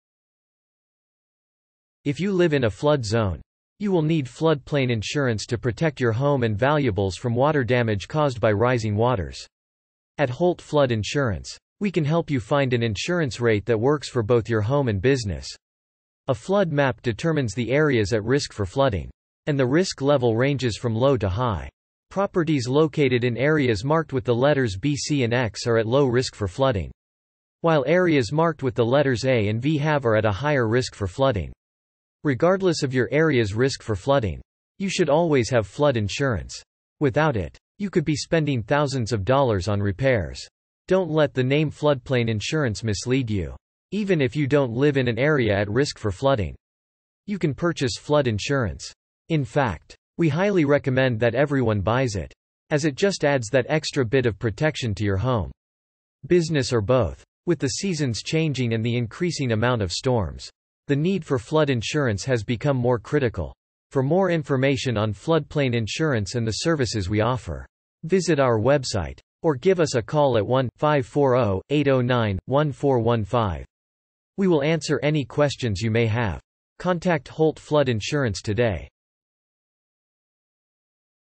floodplain insurance
If you live in a flood zone, you will need floodplain insurance to protect your home and valuables from water damage caused by rising waters. At Holt Flood Insurance, we can help you find an insurance rate that works for both your home and business.

A flood map determines the areas at risk for flooding, and the risk level ranges from low to high. Properties located in areas marked with the letters B, C, and X, are at low risk for flooding, while areas marked with the letters A and V have are at a higher risk for flooding. Regardless of your area's risk for flooding, you should always have flood insurance. Without it, you could be spending thousands of dollars on repairs.

Don't let the name floodplain insurance mislead you. Even if you don't live in an area at risk for flooding, you can purchase flood insurance. In fact, we highly recommend that everyone buys it. As it just adds that extra bit of protection to your home, business or both. With the seasons changing and the increasing amount of storms, the need for flood insurance has become more critical.

Or give us a call at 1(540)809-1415. We will answer any questions you may have. Contact Holt Flood Insurance today.